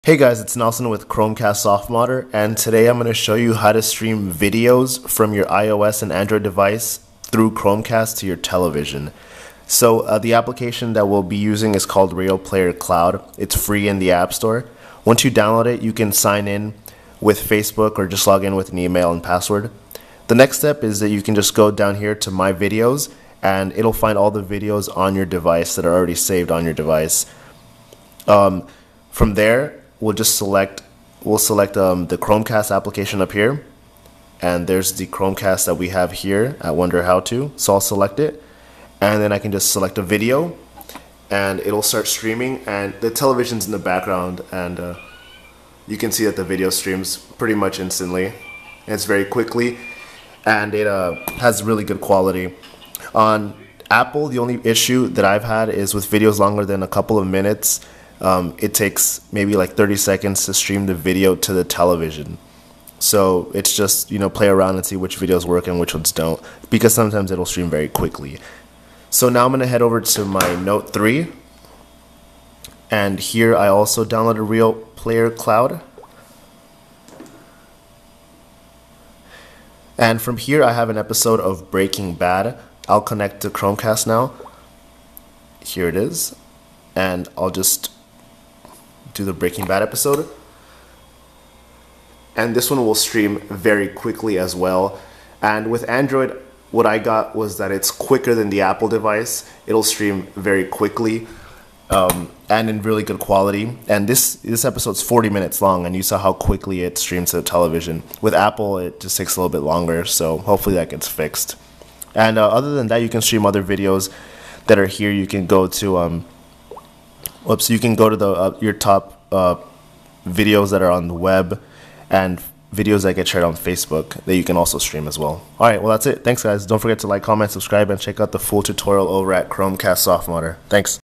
Stream Any Video on Your Android or iOS Device to Chromecast on Your TV [How-To]
Stream Videos from Phone/Tablet to Chromecast

In today's softModder guide, I'll be showing you how to play videos from your device to your Chromecast. For this guide, you'll be using the RealPlayer Cloud application, available for free on both the iOS and Android app stores. Now you can shoot over movies from your device to your Chromecast with minimal buffering.

For more information and more Chromecast tips and tricks, check out the full article over on Chromecast softModder using the link above.